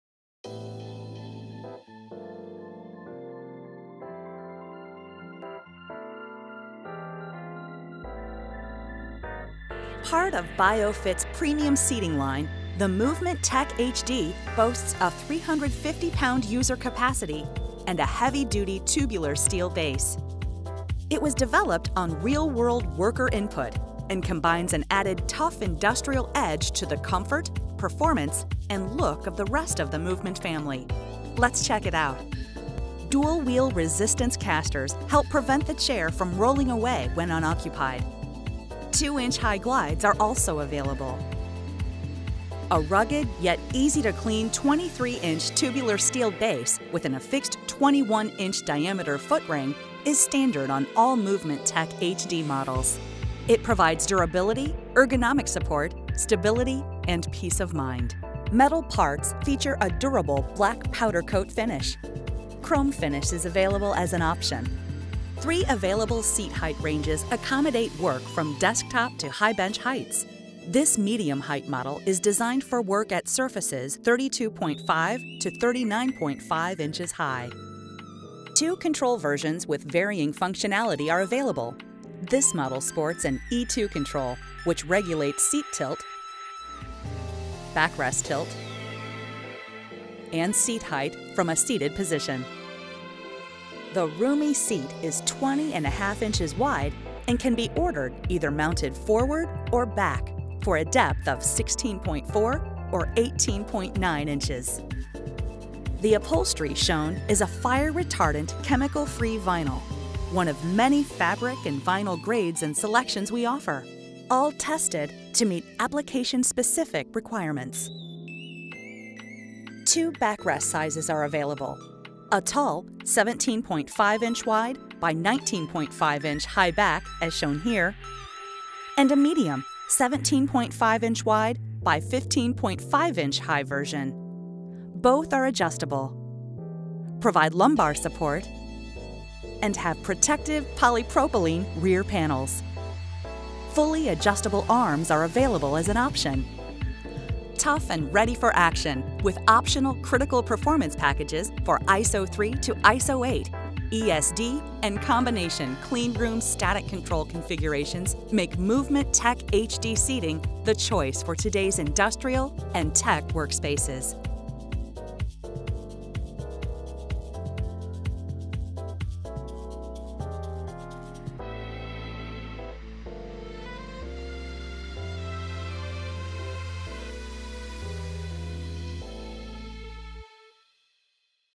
BioFit MVMT Tech HD Series Seating
Tough, tested and precision engineered to deliver ergonomic support to users in industrial and technical workplaces make the MVMT Tech HD (Heavy Duty) Series seating the go-to for today's manufacturers. Learn why with this informative video.